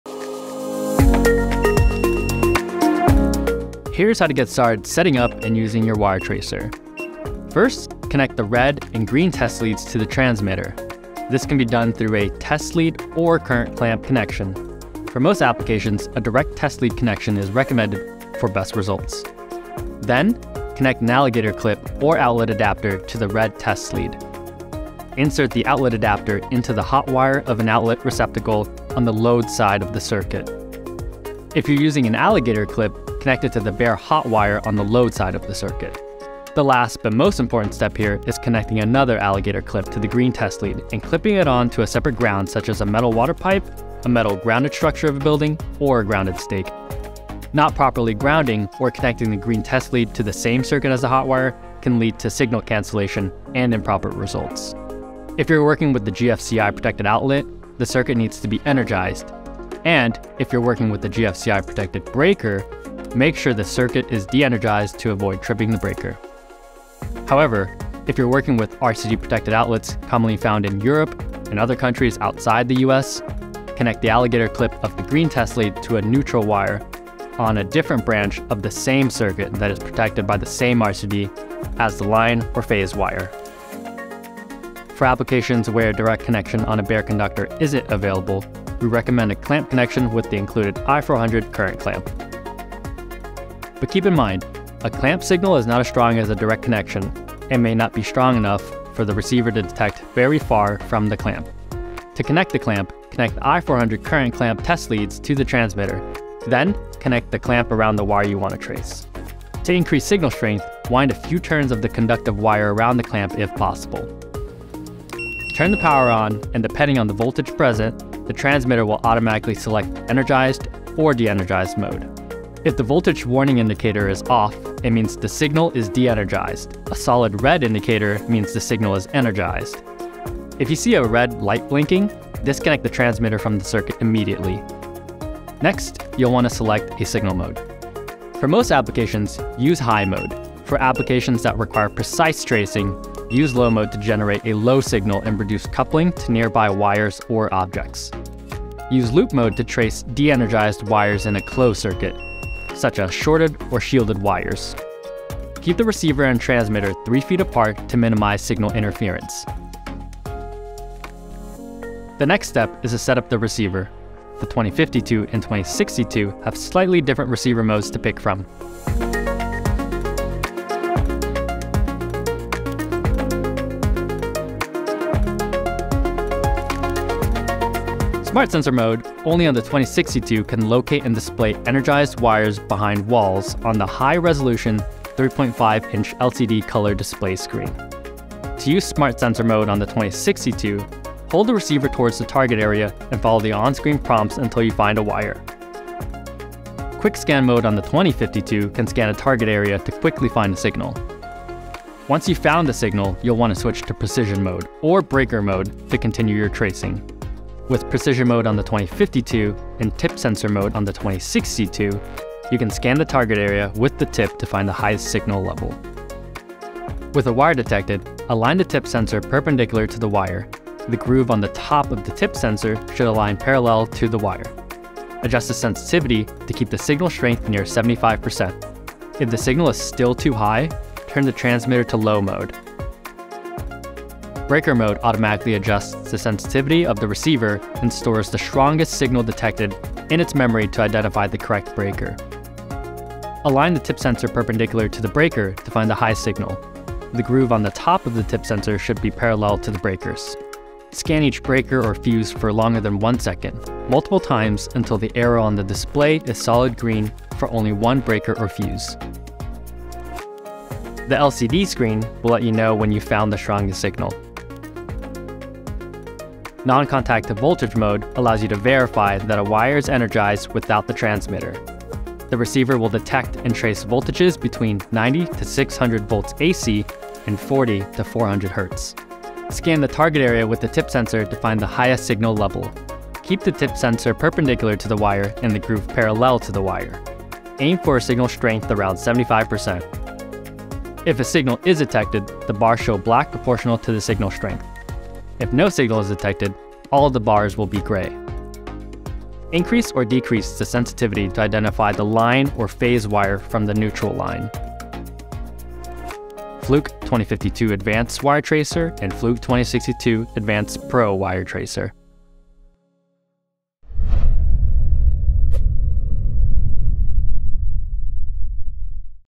How to trace wire in walls with the Fluke 2052 and 2062 Advanced Wire Tracer
Learn the step by step process for how to trace wires in walls with the Fluke 2052 and 2062 Advanced Wire Tracer